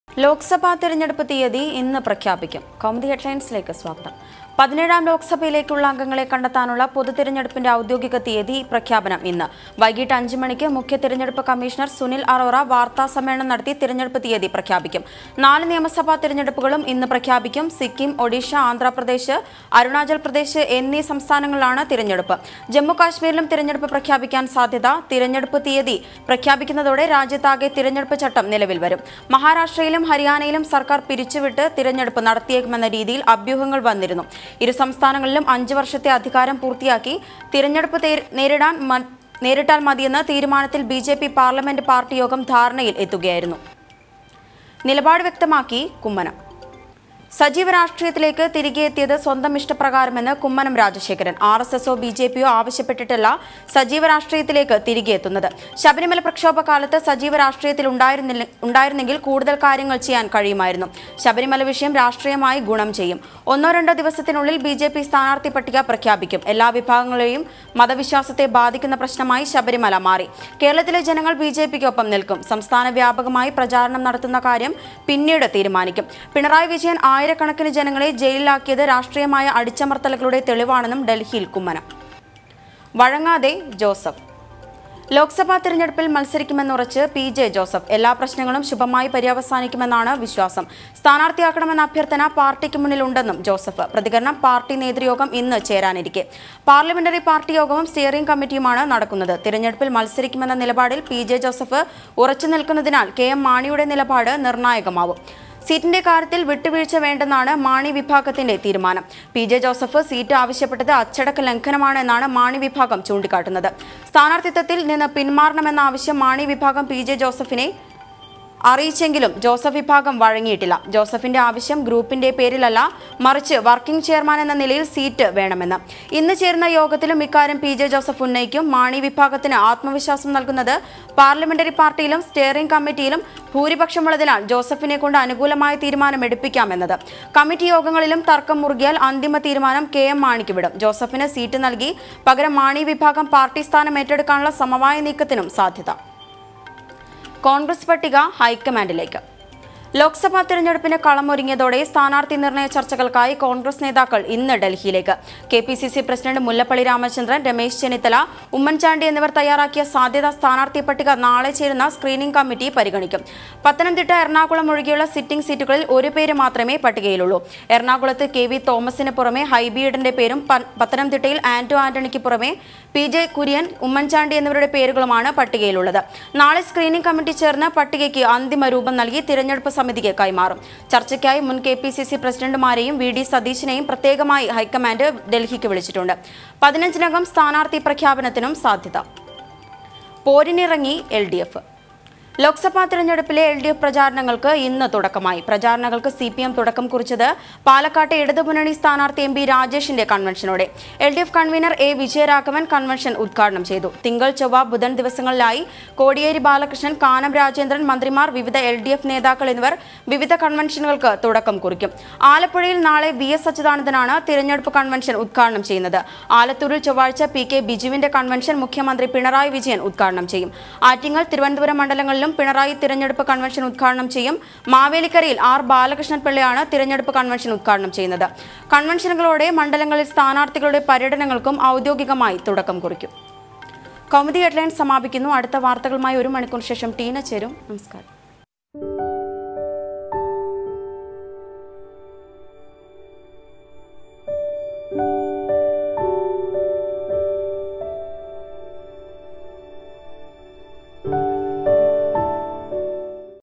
NEWS LIVE | പൊതു തിരഞ്ഞെടുപ്പിന്റെ ഔദ്യോഗിക തീയതി പ്രഖ്യാപനം ഇന്ന്
1. പതിനേഴാം ലോക്സഭയിലേക്ക് ഉള്ള അംഗങ്ങളെ കണ്ടെത്താനുള്ള പൊതു തിരഞ്ഞെടുപ്പിന്റെ ഔദ്യോഗിക തീയതി പ്രഖ്യാപനം ഇന്ന്. വൈകിട്ട് അഞ്ച് മണിക്ക് മുഖ്യ തിരഞ്ഞെടുപ്പ് കമ്മിഷണര്‍ സുനില്‍ അറോറ വാര്‍ത്താ സമ്മേളനം നടത്തി തിരഞ്ഞെടുപ്പ് തീയതി പ്രഖ്യാപിക്കും. നാല് നിയമസഭ തിരഞ്ഞെടുപ്പുകളും ഇന്ന് പ്രഖ്യാപിക്കും. സിക്കിം, ഒഡീഷ, ആന്ധ്രാപ്രദേശ്, അരുണാചല്‍ പ്രദേശ്  എന്നീ സംസ്ഥാനങ്ങളിലാണ് തിരഞ്ഞെടുപ്പ്.  ജമ്മു കാശ്മീരിലും തിരഞ്ഞെടുപ്പ് പ്രഖ്യാപിക്കാന്‍ സാധ്യത
2. തിരഞ്ഞെടുപ്പ് തീയതി പ്രഖ്യാപിക്കുന്നതോടെ രാജ്യത്താകെ തിരഞ്ഞെടുപ്പ് ചട്ടം നിലവില്‍ വരും. മഹാരാഷ്ട്രയിലും ഹരിയാനയിലും സര്‍ക്കാര്‍ പിരിച്ചു വിട്ട് തിരഞ്ഞെടുപ്പ് നടത്തിയേക്കും എന്ന രീതിയില്‍ അഭ്യൂഹങ്ങള്‍ വന്നിരുന്നു. ഇരു സംസ്ഥാനങ്ങളിലും അഞ്ച് വര്‍ഷത്തെ അധികാരം പൂര്‍ത്തിയാക്കി തിരഞ്ഞെടുപ്പ് നേരിട്ടാല്‍ മതിയെന്ന് തീരുമാനത്തില്‍ ബി.ജെ.പി പാര്‍ലമെന്റ് പാര്‍ട്ടി യോഗം ധാരണയില്‍ എത്തുക ആയിരുന്നു
3. സജീവ രാഷട്രീയത്തിലേക്ക് തിരികെ എത്തിയത് സ്വന്തം ഇഷ്ടപ്രകാരമെന്ന് കുമ്മനം രാജശേഖരന്‍.  ആര്‍.എസ്.എസോ ബി.ജെ.പിയോ ആവശ്യപ്പെട്ടിച്ചല്ല സജീവ രാഷ്ട്രീയത്തിലേക്ക് തിരികെ എത്തുന്നത്. ശബരിമല പ്രക്ഷോഭകാലത്ത് സജീവ രാഷ്ട്രീയത്തില്‍ ഉണ്ടായിരുന്നെങ്കില്‍ കൂടുതല്‍ കാര്യങ്ങള്‍ ചെയ്യാന്‍ കഴിയുമായിരുന്നു. ശബരിമല വിഷയം രാഷ്ട്രീയമായി ഗുണം ചെയ്യും.
4. ഒന്നോ രണ്ടോ ദിവസത്തിനുള്ളില്‍ ബി.ജെ.പി സ്ഥാനാര്‍ത്ഥി പട്ടിക പ്രഖ്യാപിക്കും. എല്ലാ വിഭാഗങ്ങളുടെയും മതവിശ്വാസത്തെ ബാധിക്കുന്ന പ്രശ്നമായി ശബരിമല മാറി. കേരളത്തിലെ ജനങ്ങള്‍ ബി.ജെ.പിക്കൊപ്പം നില്‍ക്കും. സംസ്ഥാന വ്യാപകമായി പ്രചാരണം നടത്തുന്ന കാര്യം പിന്നീട് തീരുമാനിക്കും. പിണറായി വിജയന്‍ ആയിരക്കണക്കിന് ജനങ്ങളെ ജയിലില്‍ ആക്കിയത് രാഷ്ട്രീയമായ അടിച്ചമര്‍ത്തലുകളുടെ തെളിവാണെന്നും ഡല്‍ഹിയില്‍ കുമ്മനം.
5. ലോക്സഭാ തിരഞ്ഞെടുപ്പില്‍ മത്സരിക്കുമെന്ന് ഉറച്ച് പി.ജെ.ജോസഫ്. എല്ലാ പ്രശ്നങ്ങളും ശുഭമായി പര്യവസാനിക്കും എന്നാണ് വിശ്വാസം. സ്ഥാനാര്‍ത്ഥി ആക്കണമെന്ന അഭ്യര്‍ത്ഥന പാര്‍ട്ടിക്ക് മുന്നില്‍ ഉണ്ടെന്നും ജോസഫ്. പ്രതികരണം, പാര്‍ട്ടി നേതൃയോഗം ഇന്ന് ചേരാനിരിക്കെ. പാര്‍ലമെന്ററി പാര്‍ട്ടി യോഗവും സ്റ്റിയറിംഗ് കമ്മിറ്റിയുമാണ് നടക്കുന്നത്. തിരഞ്ഞെടുപ്പില്‍ മത്സരിക്കും എന്ന നിലപാടില്‍ പി.ജെ ജോസഫ് ഉറച്ച് നില്‍ക്കുന്നതിനാല്‍ കെ.എം മാണിയുടെ നിലപാട് നിര്‍ണായകമാവും.
6. സീറ്റിന്റെ കാര്യത്തില്‍ വിട്ടു വീഴ്ച വേണ്ടെന്ന് ആണ് മാണി വിഭാഗത്തിന്റെ തീരുമാനം. പി.ജെ ജോസഫ് സീറ്റ് ആവശ്യപ്പെട്ടത് അച്ചടക്ക ലംഘനമാണ് എന്നാണ് മാണി വിഭാഗം ചൂണ്ടിക്കാട്ടുന്നത്. സ്ഥാനാര്‍ത്ഥിതത്വത്തില്‍ നിന്ന് പിന്മാറണം എന്ന ആവശ്യം മാണി വിഭാഗം പി.ജെ ജോസഫിനെ അറിയിച്ചെങ്കിലും ജോസഫ് വിഭാഗം വഴങ്ങിയിട്ടില്ല. ജോസഫിന്റെ ആവശ്യം, ഗ്രൂപ്പിന്റെ പേരില്‍ അല്ല മറിച്ച് വര്‍ക്കിംഗ് ചെയര്‍മാന്‍ എന്ന നിലയില്‍ സീറ്റ് വേണമെന്ന്
7. ഇന്ന് ചേരുന്ന യോഗത്തിലും ഇക്കാര്യം പി.ജെ ജോസഫ് ഉന്നയിക്കും. മാണി വിഭാഗത്തിന് ആത്മവിശ്വാസം നല്‍കുന്നത് പാര്‍ലമെറന്റി പാര്‍ട്ടിയിലും സ്റ്റിയറിംഗ് കമ്മിറ്റിയിലും ഭൂരിപക്ഷമുള്ളതിനാല്‍ ജോസഫിനെ കൊണ്ട് അനുകൂലമായ തീരുമാനം എടുപ്പിക്കാം എന്നത്. കമ്മിറ്റി യോഗങ്ങളിലും തര്‍ക്കം മുറുകിയാല്‍ അന്തിമ തീരുമാനം കെ.എം മാണിക്ക് വിടും. ജോസഫിന് സീറ്റ് നല്‍കി പകരം മാണി വിഭാഗം പാര്‍ട്ടി സ്ഥാനം ഏറ്റെടുക്കാനുള്ള സമവായ നീക്കത്തിനും സാധ്യത
8. ലോക്സഭ തിരഞ്ഞെടുപ്പിന് കളം ഒരുങ്ങിയതോടെ സ്ഥാനാര്‍ത്ഥി നിര്‍ണയ ചര്‍ച്ചകള്‍ക്കായി കോണ്‍ഗ്രസ് നേതാക്കള്‍ ഇന്ന് ഡല്‍ഹിയിലേക്ക്. കെ.പി.സി.സി പ്രസിഡന്റ് മുല്ലപ്പള്ളി രാമചന്ദ്രന്‍, രമേശ് ചെന്നിത്തല, ഉമ്മന്‍ ചാണ്ടി എന്നിവര്‍ തയ്യാറാക്കിയ സാധ്യത സ്ഥാനാര്‍ത്ഥി പട്ടിക നാളെ ചേരുന്ന സക്രീനിംഗ് കമ്മിറ്റി പരിഗണിക്കും. പത്തനംതിട്ട, എറണാകുളം ഒഴികെയുള്ള സിറ്റിംഗ് സീറ്റുകളില്‍ ഒരു പേര് മാത്രമേ പട്ടികയിലുള്ളൂ.
9. എറണാകുളത്ത് കെ.വി. തോമസിന് പുറമേ ഹൈബി ഈഡന്റെ പേരും പത്തനംതിട്ടയില്‍ ആന്റോ ആന്റണിക്ക് പുറമേ പി.ജെ. കുര്യന്‍, ഉമ്മന്‍ചാണ്ടി എന്നിവരുടെ പേരുകളുമാണ് പട്ടികയില്‍ ഉള്ളത്. നാളെ സ്‌ക്രീനിംഗ് കമ്മറ്റി ചേര്‍ന്ന് പട്ടികയ്ക്ക് അന്തിമ രൂപം നല്‍കി തിരഞ്ഞെടുപ്പ് സമിതിക്ക് കൈമാറും. ചര്‍ച്ചയ്ക്കായി മുന്‍ കെ.പി.സി.സി പ്രസിഡന്റുമാരെയും വി.ഡി സതീശനെയും പ്രത്യേകമായി ഹൈക്കമാന്‍ഡ് ഡല്‍ഹിക്ക് വിളിച്ചിട്ടുണ്ട്. 15നകം സ്ഥാനാര്‍ഥി പ്രഖ്യാപനത്തിനും സാധ്യത
10. ലോക്സഭ തിരഞ്ഞെടുപ്പിലെ എല്‍.ഡി.എഫ്  പ്രചാരണങ്ങള്‍ക്ക് ഇന്ന് തുടക്കമാകും. പ്രചാരണങ്ങള്‍ക്ക് സി.പി.എം തുടക്കം കുറിക്കുന്നത്, പാലക്കാട്ടെ ഇടത് മുന്നണി സ്ഥാനാര്‍ത്ഥി എം.ബി രാജേഷിന്റെ കണ്‍വെന്‍ഷനോടെ. എല്‍.ഡി.എഫ് കണ്‍വീനര്‍ എ. വിജയരാഘവന്‍ കണ്‍വെന്‍ഷന്‍ ഉദ്ഘാടനം ചെയ്യും. തിങ്കള്‍, ചൊവ്വ, ബുധന്‍ ദിവസങ്ങളിലായി കോടിയേരി ബാലകൃഷ്ണന്‍, കാനം രാജേന്ദ്രന്‍, മന്ത്രിമാര്‍, വിവിധ എല്‍.ഡി.എഫ് നേതാക്കള്‍ എന്നിവര്‍ വിവിധ കണവെന്‍ഷനുകള്‍ക്ക് തുടക്കം കുറിക്കും.
11. ആലപ്പുഴയില്‍ നാളെ വി എസ് അച്യുതാനാന്ദനാണ് തിരഞ്ഞെടുപ്പ് കണ്‍വെന്‍ഷന്‍ ഉദ്ഘാടനം ചെയ്യുന്നത്. ആലത്തൂരില്‍ ചൊവ്വാഴ്ച പി. കെ ബിജുവിന്റെ കണ്‍വെന്‍ഷന്‍ മുഖ്യമന്ത്രി പിണറായി വിജയന്‍ ഉദ്ഘാടനം ചെയ്യും. ആറ്റിങ്ങല്‍, തിരുവനന്തപുരം മണ്ഡലങ്ങളിലും പിണറായി തിരഞ്ഞെടുപ്പ് കണ്‍വെന്‍ഷന്‍ ഉദ്ഘാടനം ചെയ്യും. മാവേലിക്കരയില്‍ ആര്‍.ബാലകൃഷ്ണപ്പിള്ള ആണ് തെരഞ്ഞെടുപ്പ് കണ്‍വെന്‍ഷന്‍ ഉദ്ഘാടനം ചെയ്യുന്നത്. കണ്‍വെന്‍ഷനുകളോടെ മണ്ഡലങ്ങളില്‍ സ്ഥാനാര്‍ത്ഥികളുടെ പര്യടനങ്ങള്‍ക്കും ഔദ്യോഗിക തുടക്കം കുറിക്കും